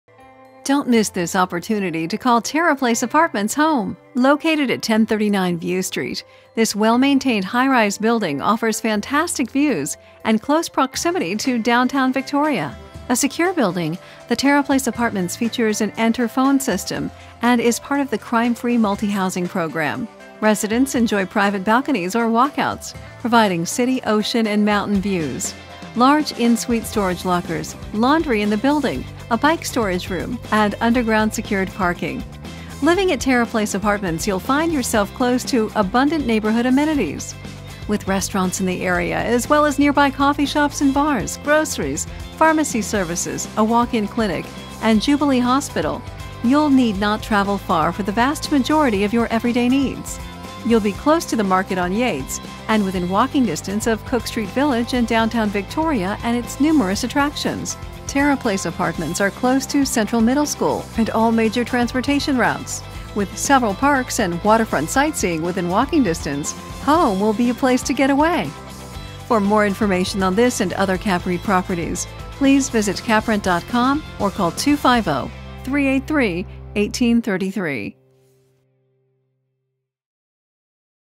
Victoria Apartments for Rent Video - 1039 View St
For more information on these apartments for rent in victoria visit our web page.

Tara Place Apartments
1039 View Street, Victoria, BC

A beautifully maintained concrete high rise in an ideal location for an apartment for rent in Victoria. Tara Place is close to all necessary services and is an easy walk to the vibrant downtown Victoria core area. Don't miss the opportunity to call this site your next home.